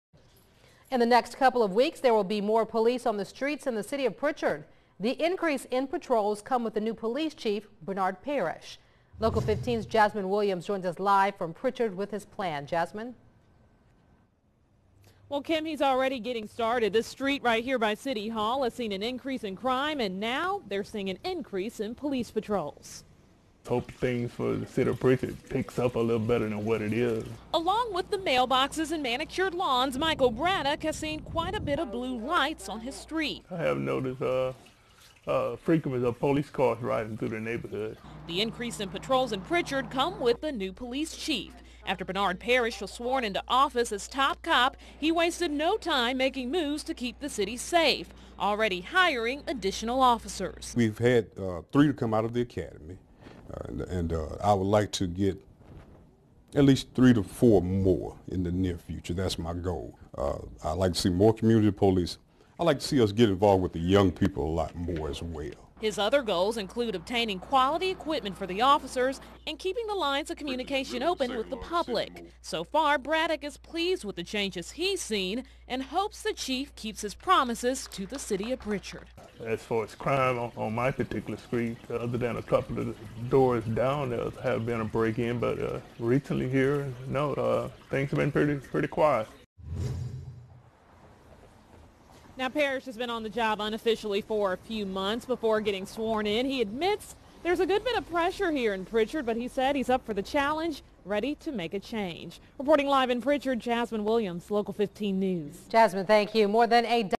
Chief: Prichard Police Dept. Needs More Officers
PRICHARD, Ala. (WPMI) After Bernard Parrish was sworn into office as Prichard's Police Chief Thursday night, he wasted no time making moves to keep the city safe. He's already hired additional officers and spent most of Friday evaluating the department.

"I've had three come out of the academy, I'd like to get three or four more officers in the near future, that's my goal," Parrish said, "I'd like to see more community policing and I'd like to see us get involved with the young people as well."

Parrish said he wants to get quality equipment for his officers and keep the lines of communication open between police and the public.

So far, some neighborhoods have already noticed the change. Michael Braddock said he's seen more police on the streets where he live and he hopes Parrish keeps it up.

"I've noticed police cars riding through the neighborhood," Braddock said, "as far as crime on my particular street, things have been pretty quiet."